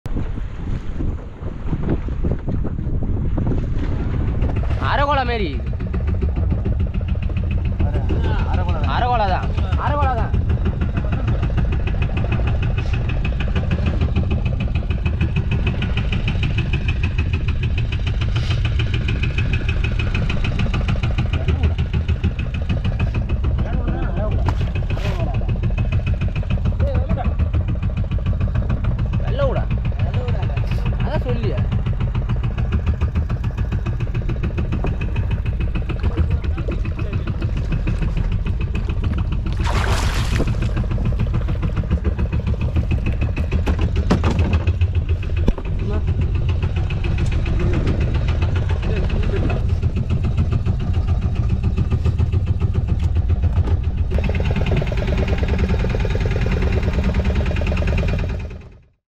Catching Giant Barracuda Fish in the Deep Sea
We Caught Giant Barracuda Fish in the Deep Sea